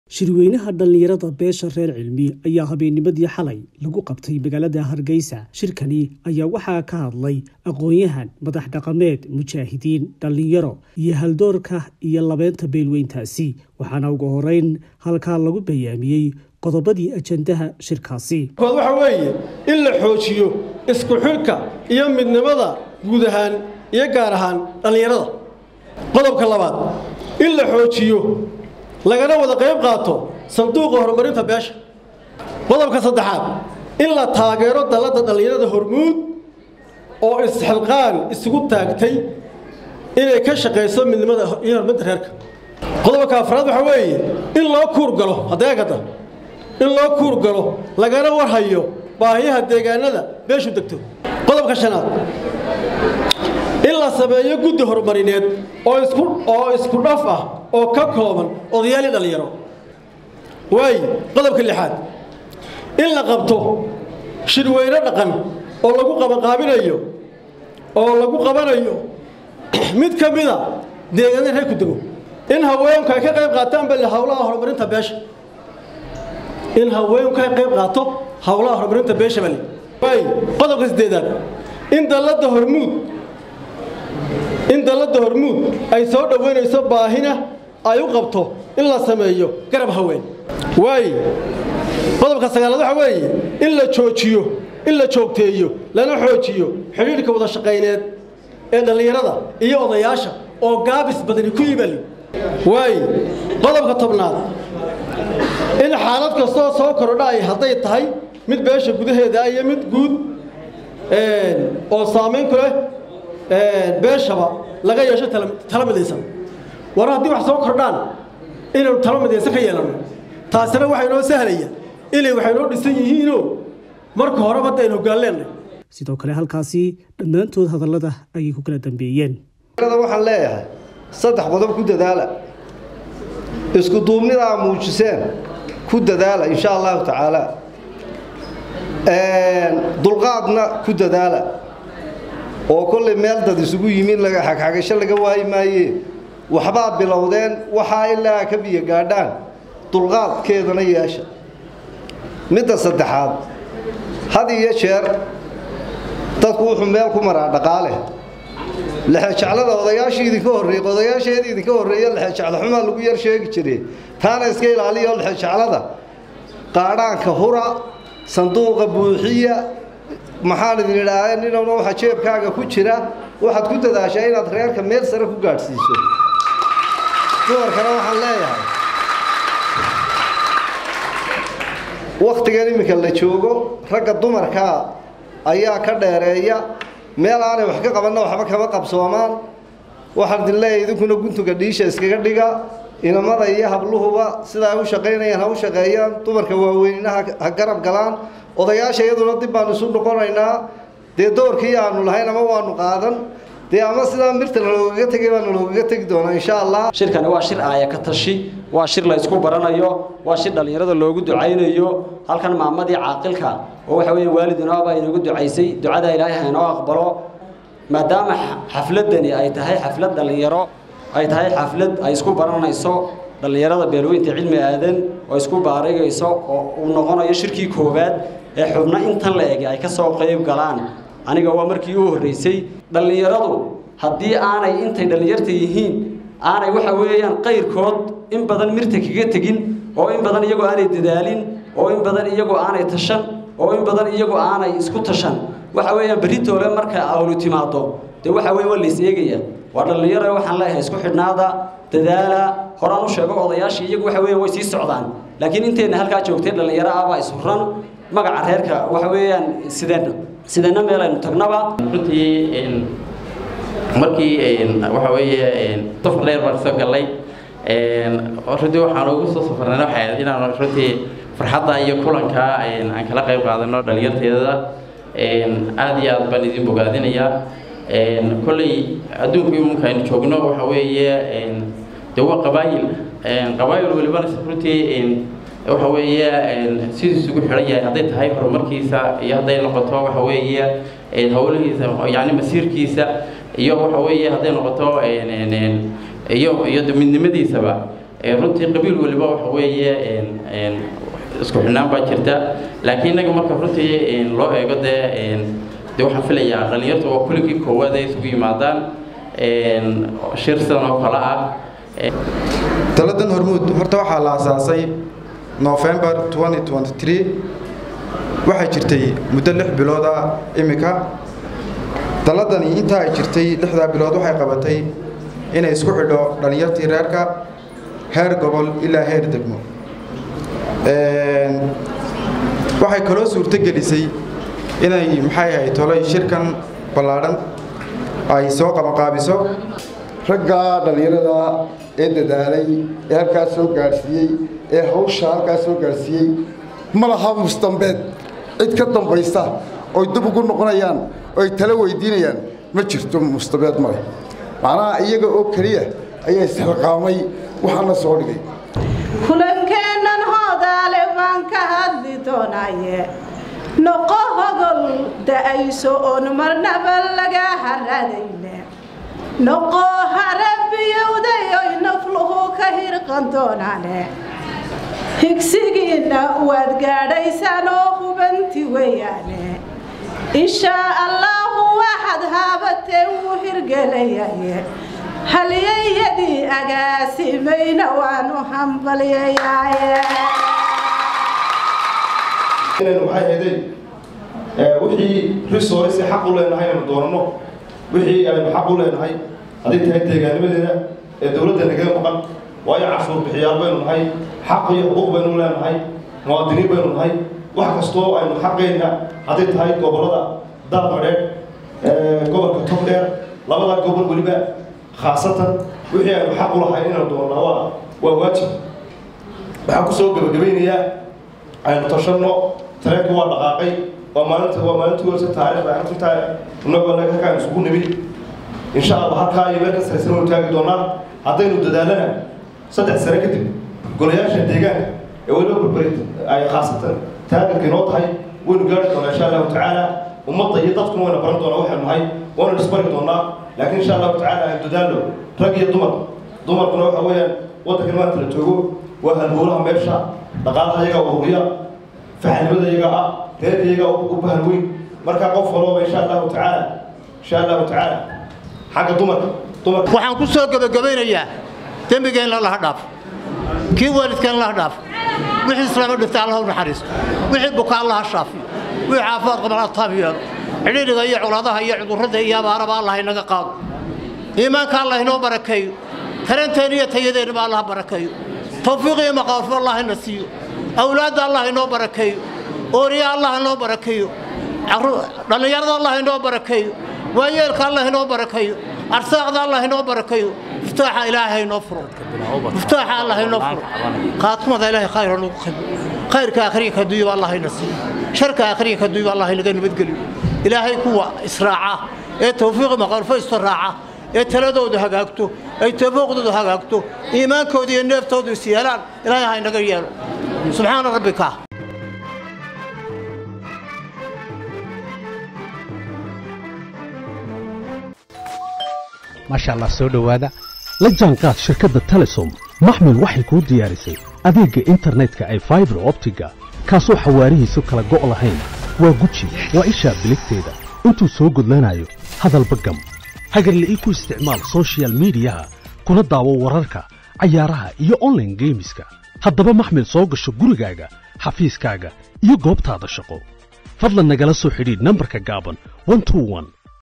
Shirka Dhallinyaradda & Haldoorka Beesha Reer Cilmi (Habaryoonis) oo xalay ka qabsoomey magaalada..
Shirka Dhallinyaradda & Haldoorka Beesha Reer Cilmi (Habaryoonis) oo xalay ka qabsoomey magaalada Hargeysa.